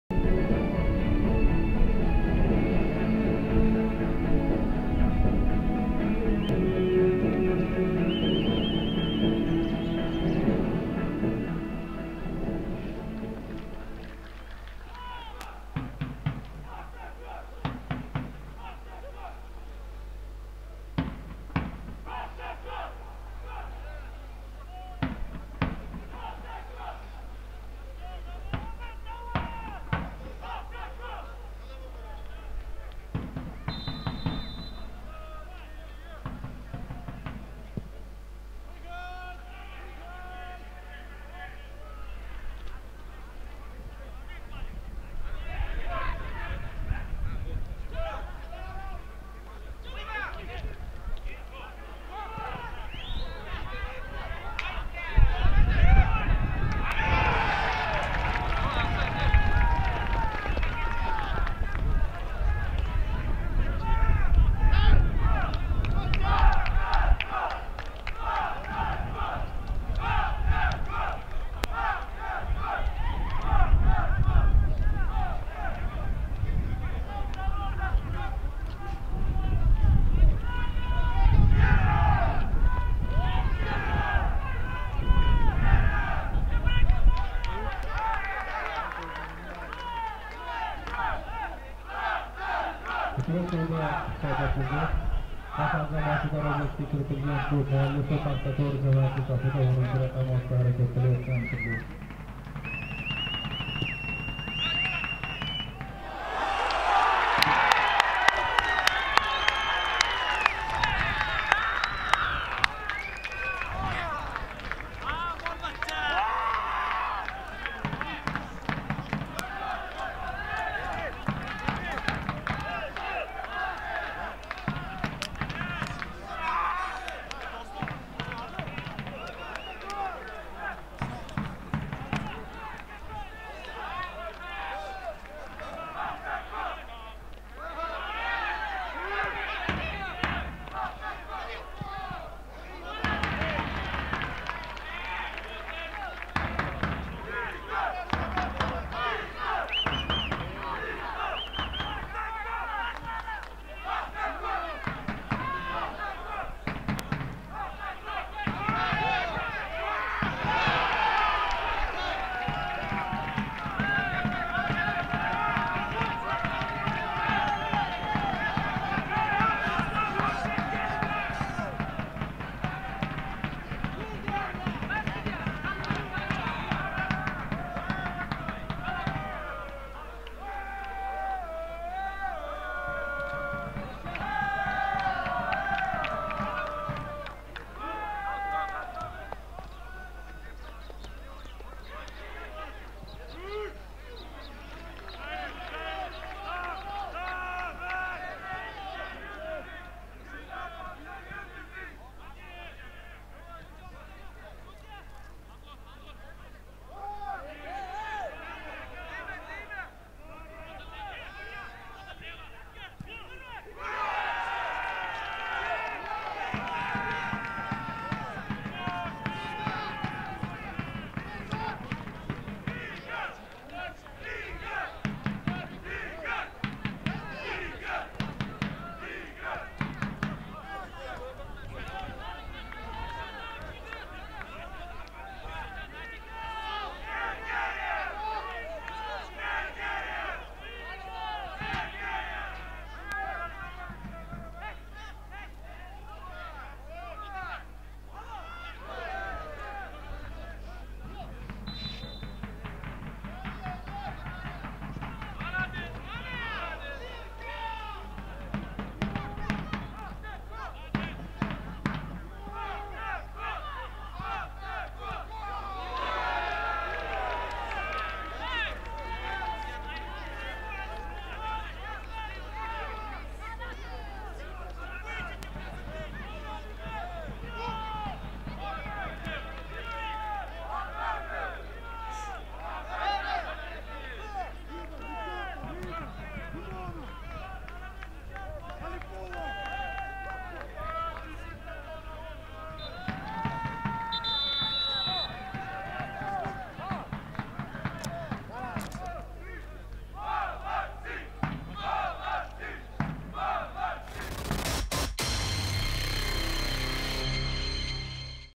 Coca Cola Superliga. 21-tur MASH'AL — PAXTAKOR 1:4
Coca Cola Superliga. 21-tur

MASH'AL — PAXTAKOR 1:4

⚽️ Dragan Cheran, 5 (0:1), Sherzod Temirov, 13 (1:1), Dostonbek Hamdamov, 31 (1:2), Igor Sergeyev, 73 (1:3), Diyor Xolmatov, 90+3 (1:4)